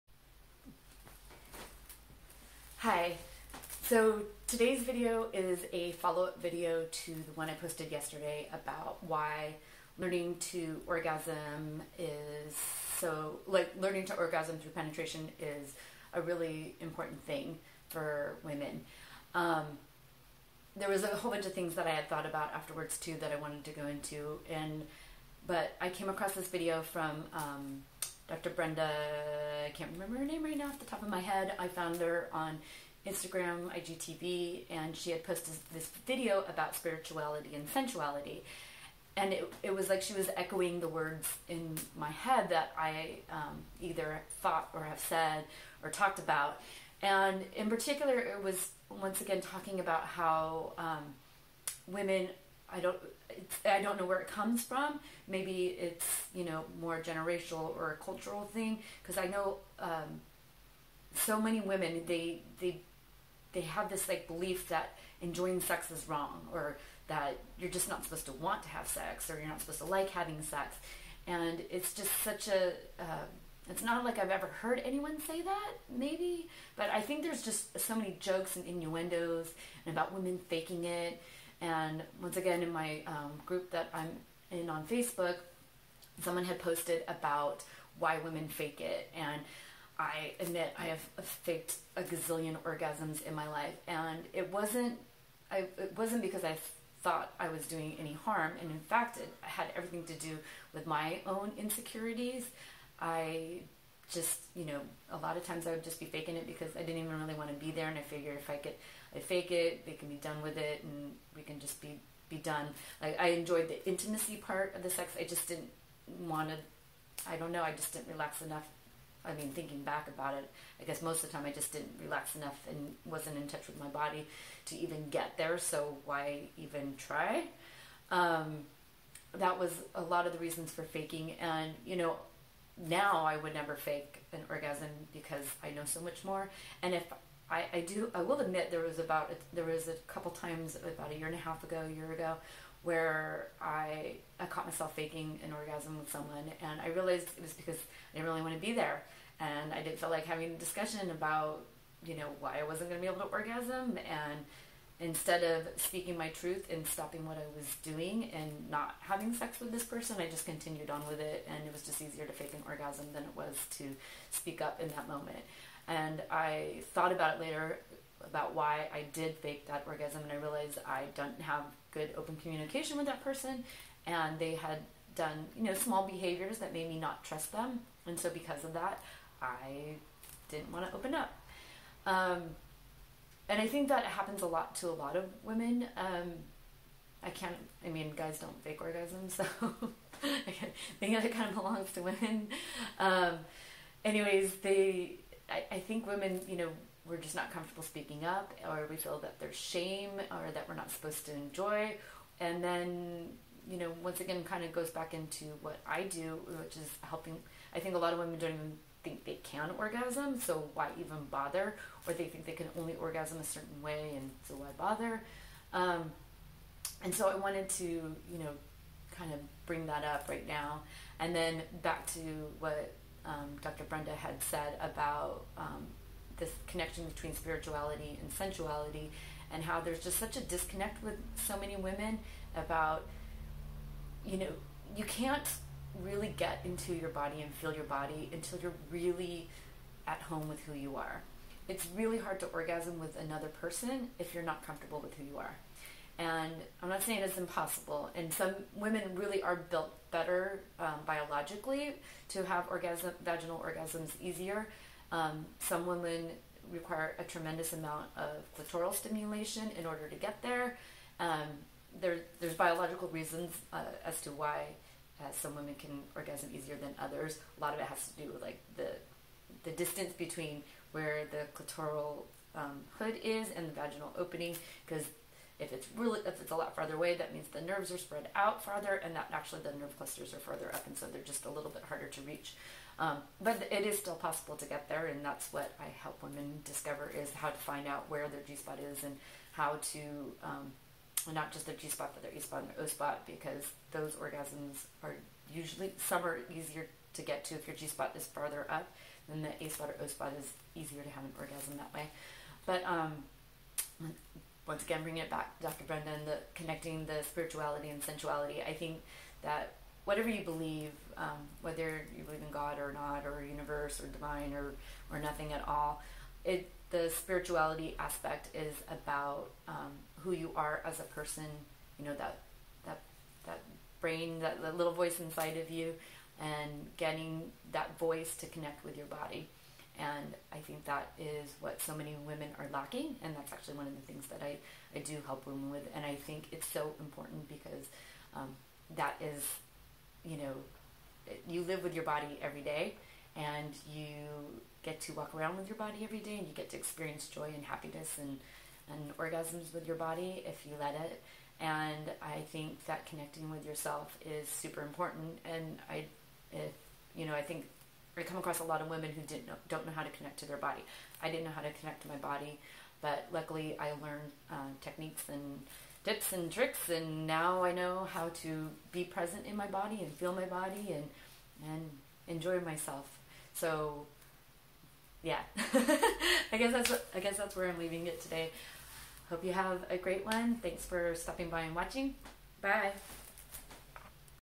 Faking orgasms, connecting spirituality to sensuality, and getting in your body
Brief discussion on:
• Why women fake orgasms
• Importance of connecting your spirituality to your sensuality
• Connecting your mind to your body